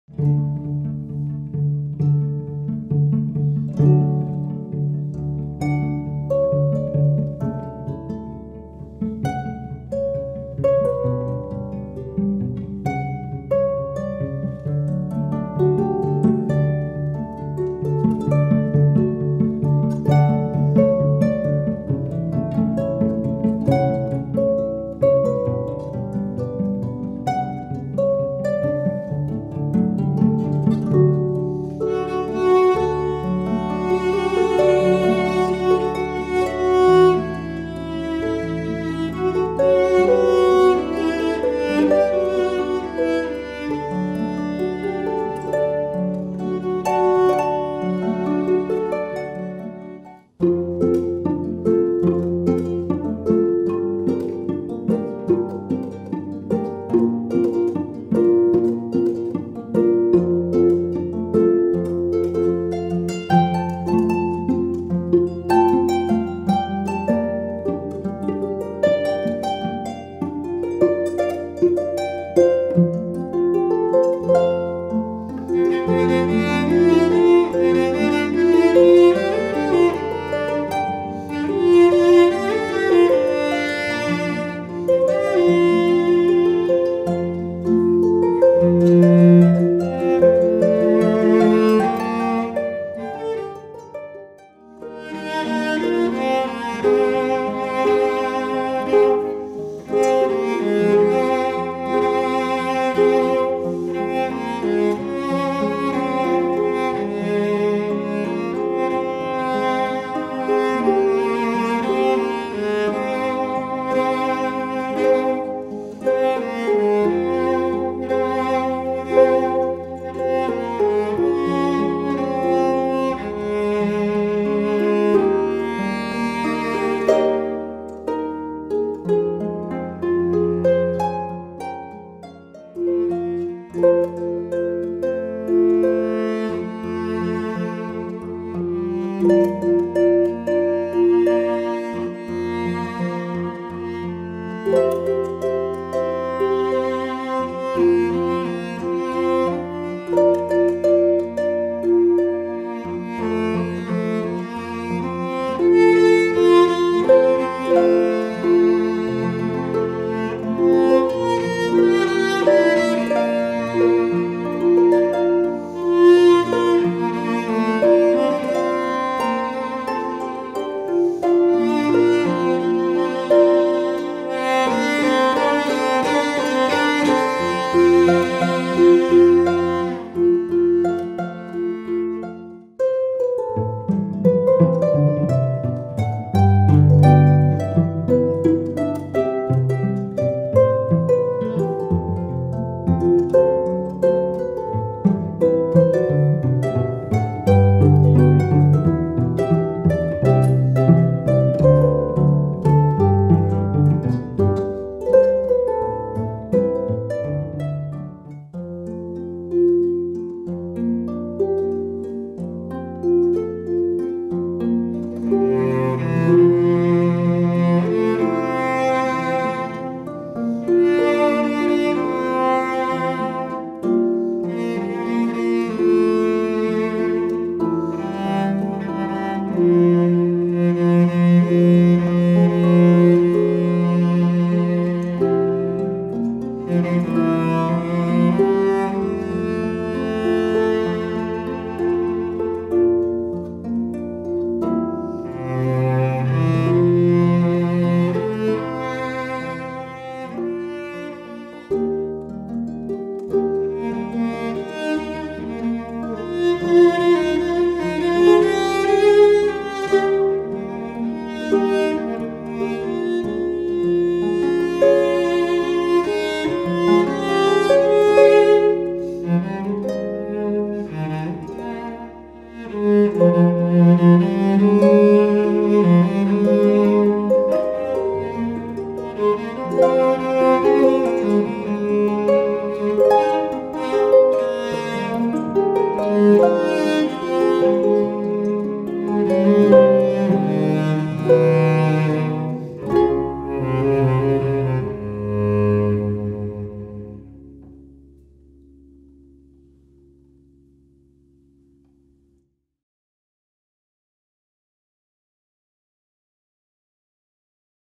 Manchester Harp and Cello Duo // Pop Medley // Book at Warble Entertainment
Whether you're looking for something elegant and beautiful, or an interesting twist on your favourite pop tunes, the stunning sound of The Manchester Harp and Cello Duo provides the perfect musical backdrop for any wedding, function or party.